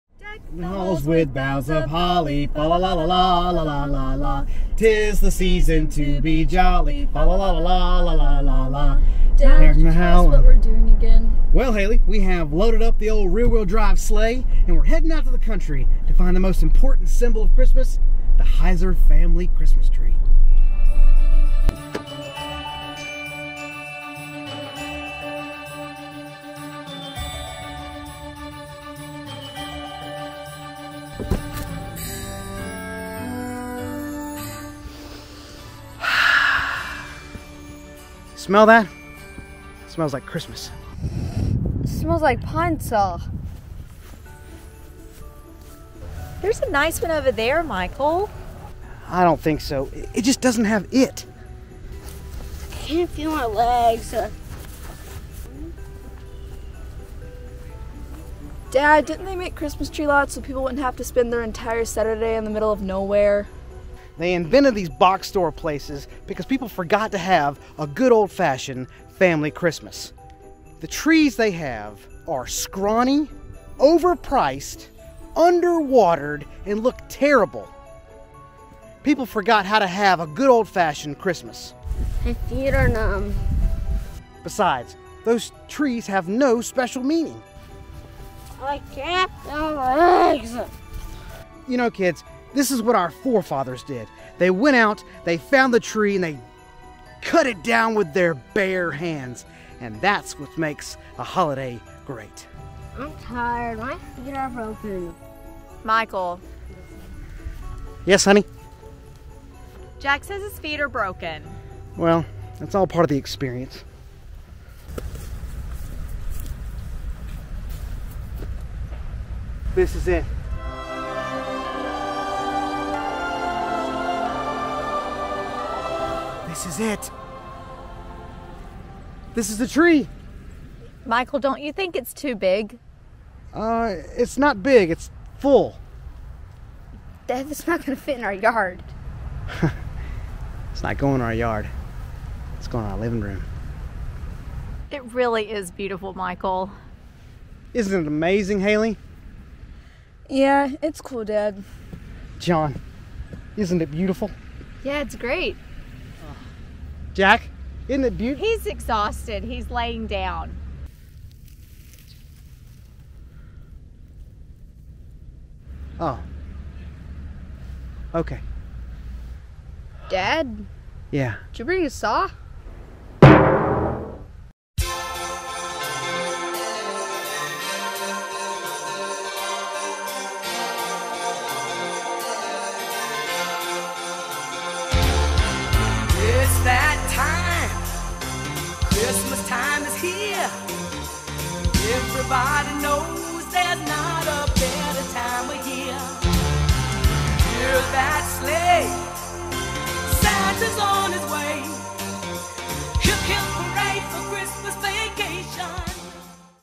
National Lampoon's Christmas Vacation parody - The 2020 Heiser Family Christmas Tree -
We love going to the local Christmas tree farm every year. This year we thought we would do our best to recreate one of our favorite movies, "National Lampoon's Christmas Vacation". I hope you enjoy the video and God Bless your home & Merry Christmas from my family to yours.

Thank you to Webster's Christmas tree farm for letting us shoot our little parody.